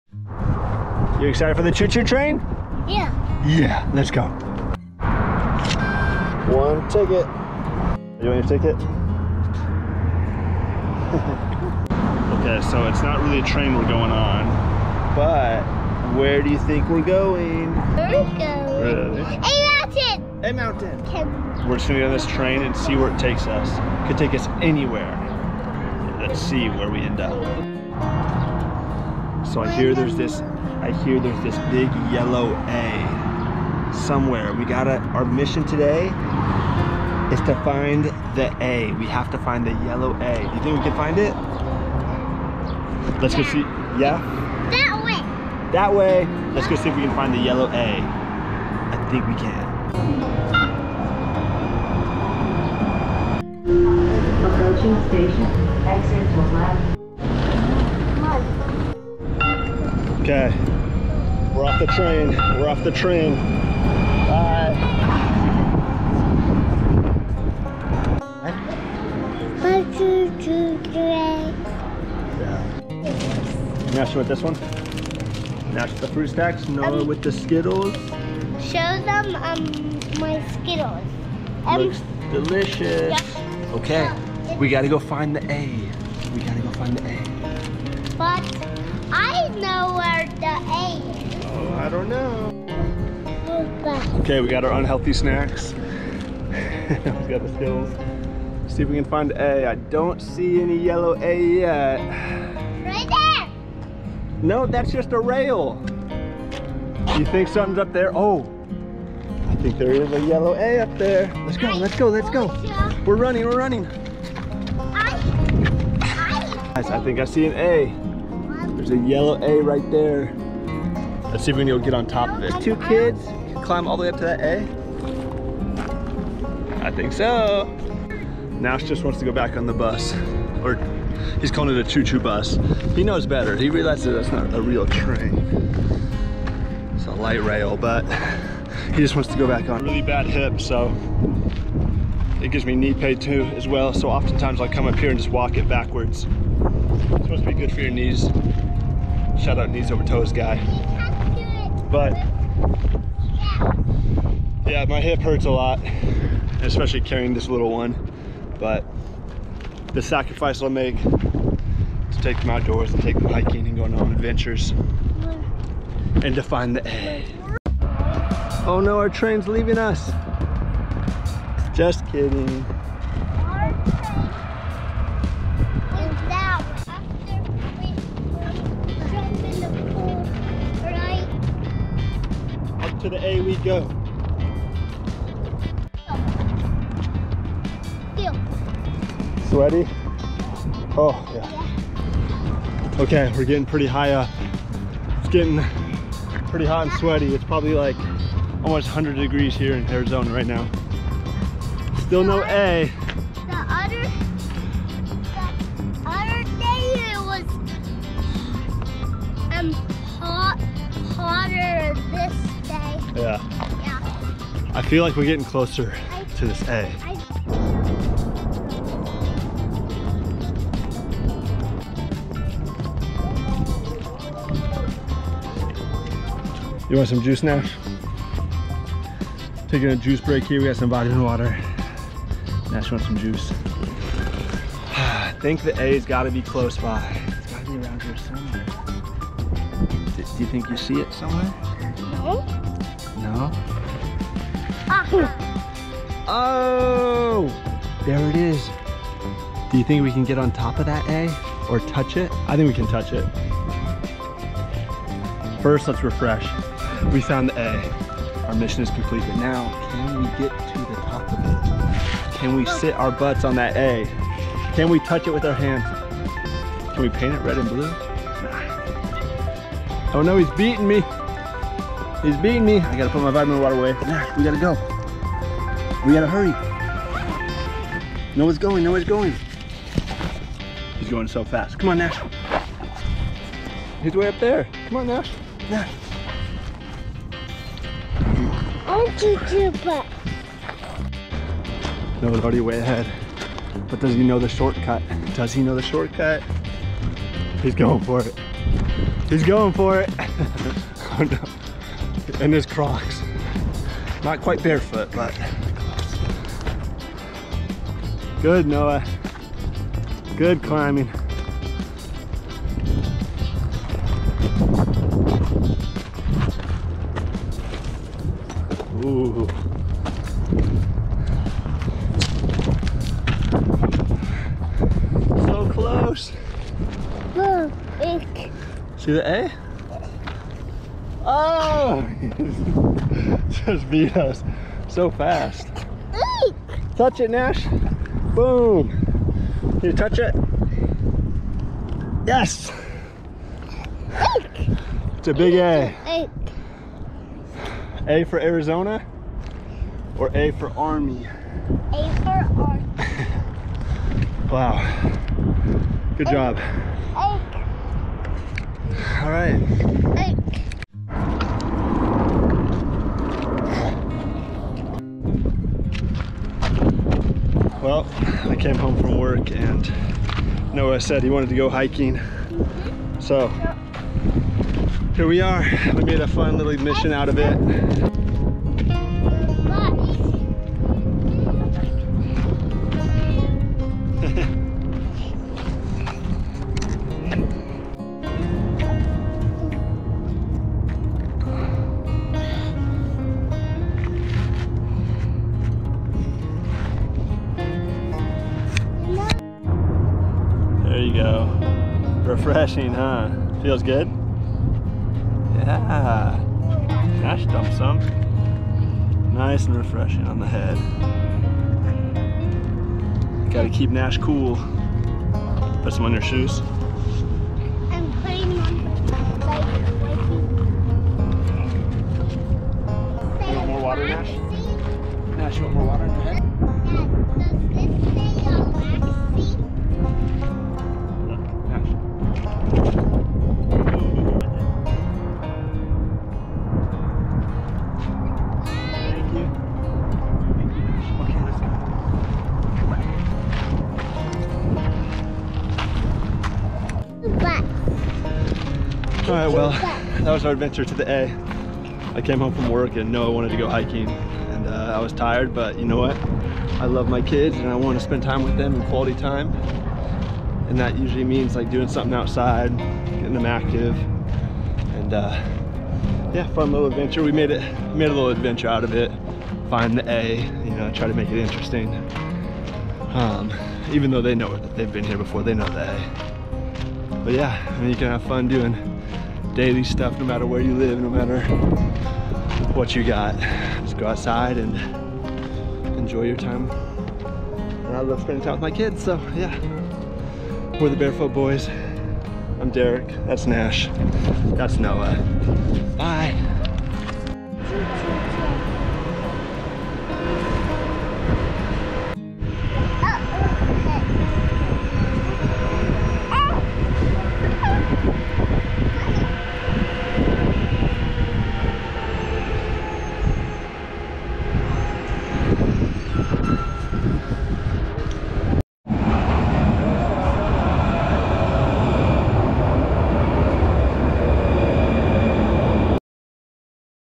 Father and Sons Adventure in Town! Can we find the "A" ?
WE ARE THE BAREFOOT BOYS!
Our family loves to spend time outdoors and find creative ways to enjoy life.

In this video I get home from work and my son Noah says he wants to go hiking. I get the idea to take the train and bring Nash! Noah's younger brother. It is his first time taking the train in the city and he wanted to ride it all day. We go on an Adventure of finding the A on A Mountain in Tempe, AZ.